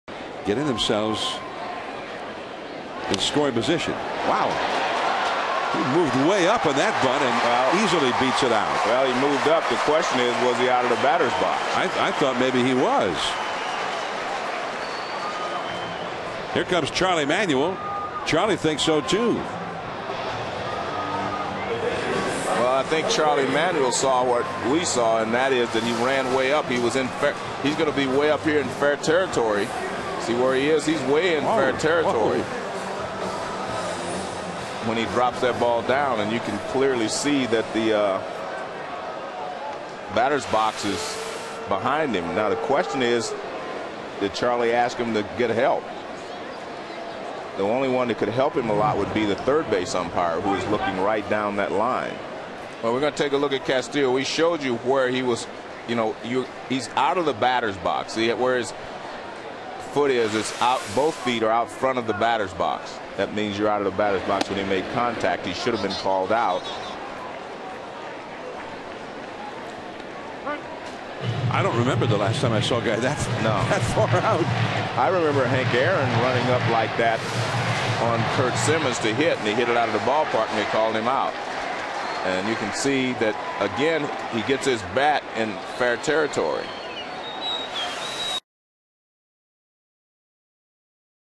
Castillo's disputed bunt
PHI@NYM: Castillo reaches on a bunt, Manuel disputes